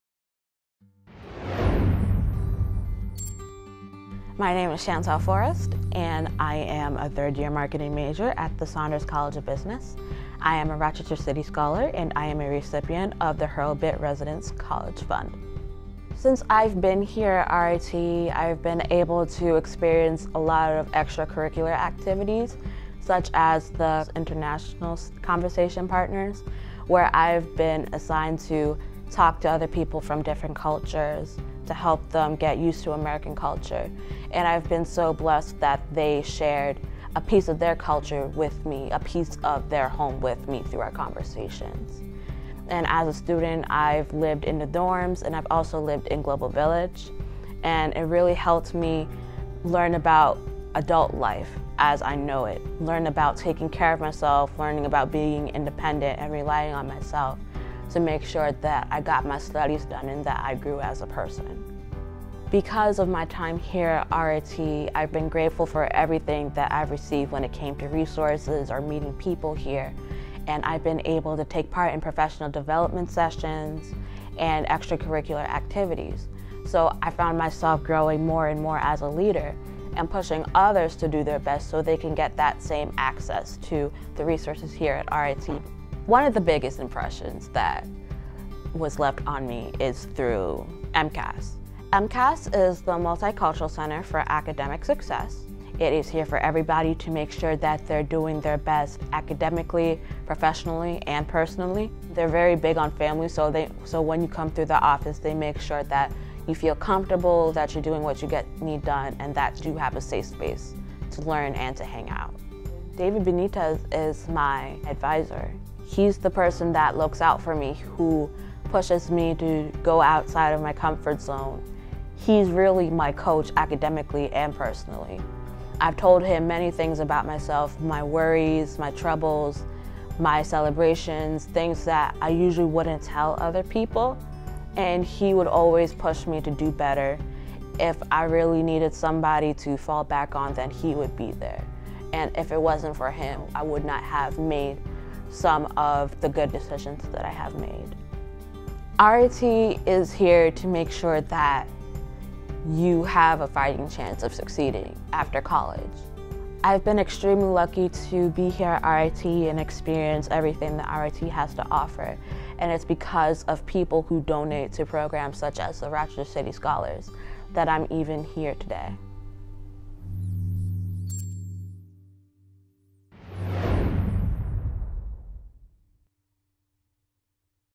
RIT Gifts in Action - Shantel Forrest
RIT gifts put student dreams into action. Hear from Shantel Forrest, the RIT/Rochester City Scholars Hurlbut Residence Fund Scholar about how the scholarship gifts to this important program have impacted her studies and her future.